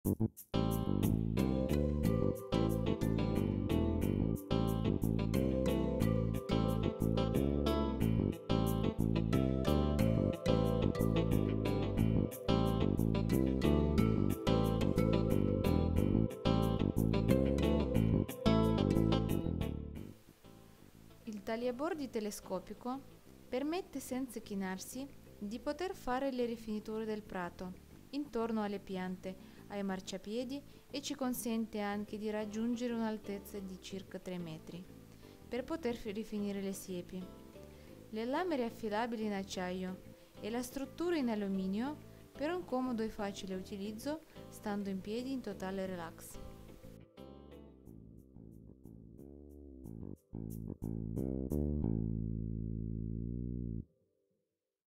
Taglia bordi telescopico
Il taglia bordi telescopico permette, senza chinarsi, di poter fare le rifiniture del prato intorno piante e marciapiedi.
Consente anche di raggiungere un'altezza di circa 3 metri per poter rifinire le siepi.
Le lame riaffilabili in acciaio e la struttura in alluminio per un comodo e facile utilizzo stando in piedi  il totale relax.